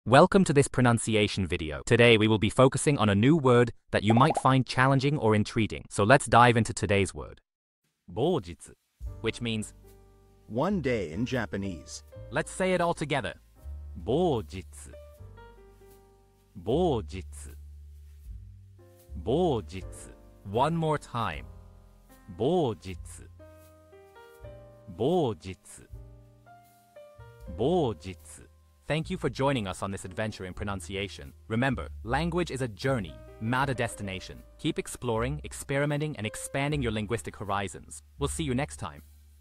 How to pronounce boujitsu | 某日 (One day in Japanese)
Master the Pronunciation of 'boujitsu | 某日' - which means : One day in Japanese 📝 with @PronunciationJapanese 🎓🗣️ - [Your Guide to Japanese]

Learn to pronounce 'boujitsu' perfectly in Japanese with our easy and engaging tutorial by @PronunciationJapanese! 📖📽️ In this comprehensive video, we provide you with native speaker examples 🗣️👂 and phonetic breakdowns 🎵 to ensure you get the pronunciation just right.